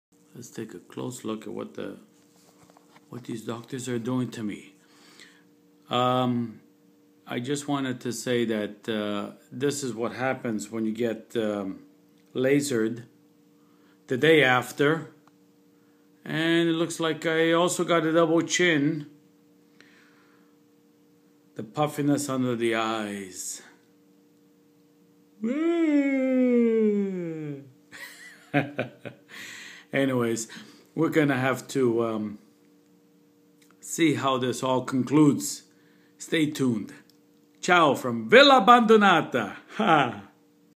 The Day After !
One day after laser surgery ...puffiness , double chin and positive attitude ! Stay tooned folks  !